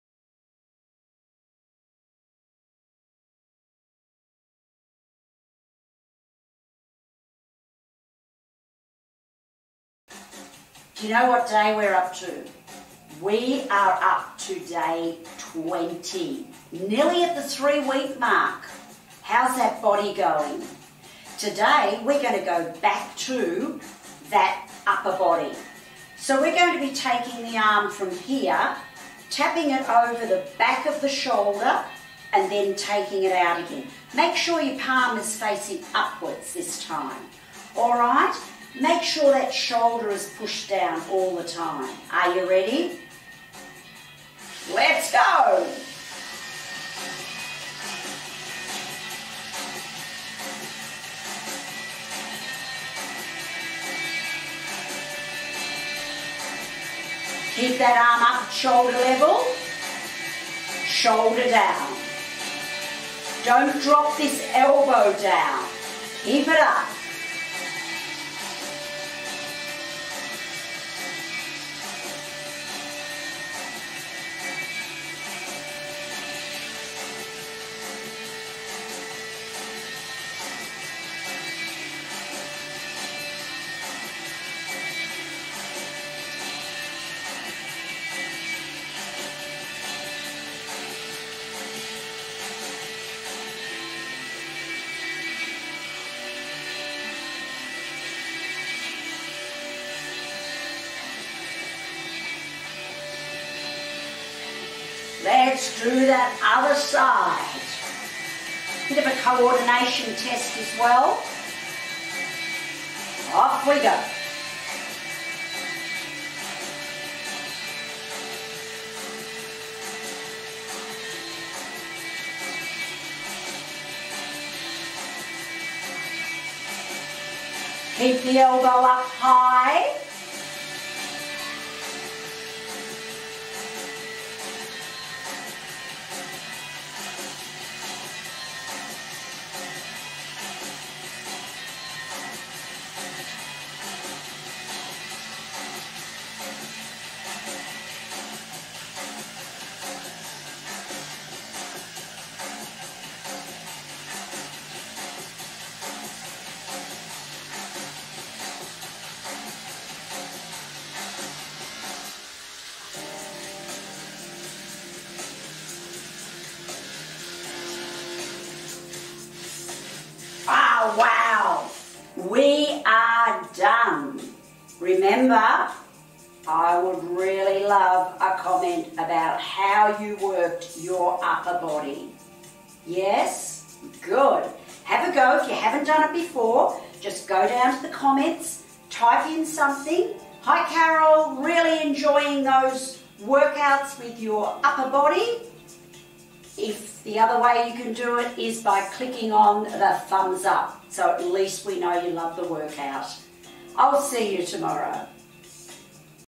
Day 20: The 30 Day Toothbrush Challenge for older adults & seniors - brushing & exercising together!
Do you brush your teeth each day? Maybe even twice a day? Why not put those two minutes to even better use by adding in an exercise that will challenge and improve your balance, mobility and strength, all while you're doing your teeth!? If you’re an older adult or senior who wants to keep getting stronger, we’ve got just for you 30 exercises for 30 days - are you up for the challenge of a mini-workout each day?

Get your toothbrush and toothpaste ready, then press play and join Carol as she brushes her teeth and gives you a new exercise each day to do with her. She'll work your arms and legs, and challenge your co-ordination and balance as well. She'll also have a laugh when she forgets to brush her teeth too!

If you enjoyed the challenge, click on the thumbs up and like this video.

At Third Age Fitness we are the home of older adults, where age is no barrier to exercise!